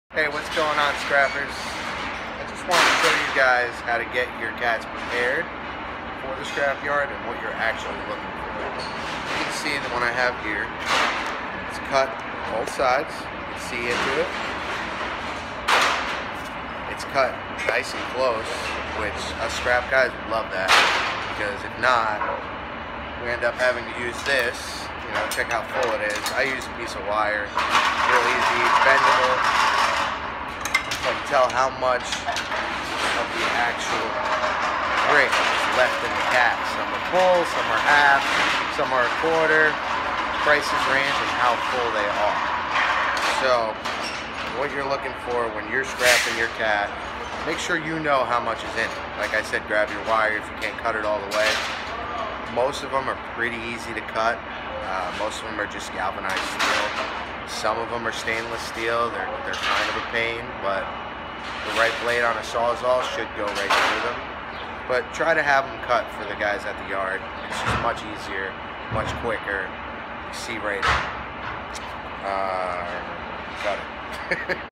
How To Prepare Your Catalytic Converters for Scrap
Report Scrap Prices - Joe walks you through the simple ways to prepare your catalytic converter for scrap including where to exhaust pipes should be cut off, also how to check how full your cat is, and what your scrap yard is going to look for. Remember every catalytic converter and scrap yard is different so make sure you check what your yard accepts and ask them how they want them prepared.